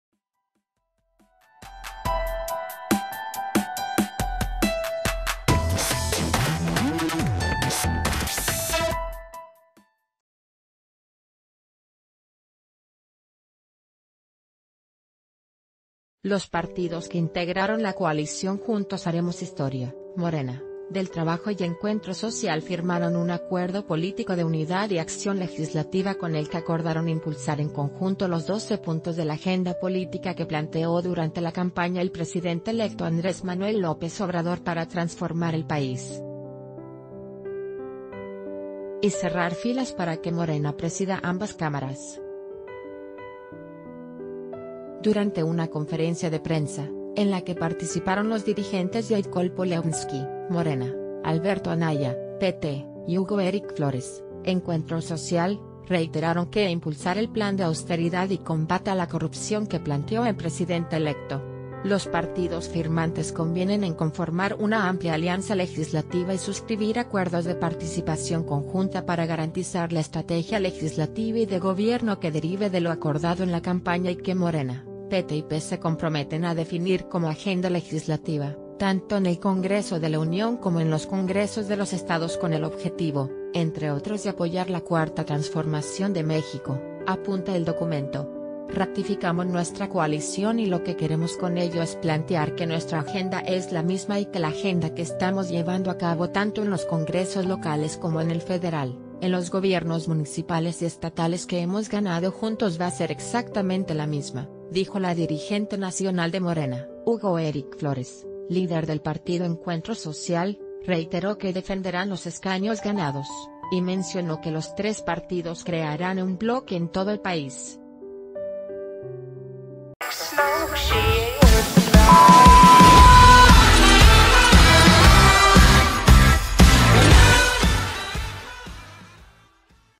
Morena, PT y PES impulsarán agenda política de AMLO
Los partidos que integraron la coalición Juntos haremos historia, Morena, del Trabajo y Encuentro Social firmaron un Acuerdo Político de Unidad y Acción Legislativa con el que acordaron impulsar en conjunto los 12 puntos de la agenda política que planteó durante la campaña el presidente electo André...